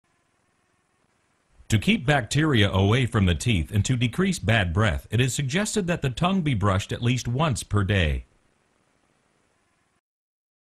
Tongue brushing
Learn how keeping your tongue clean can help your over all oral hygiene as well as give you fresh breath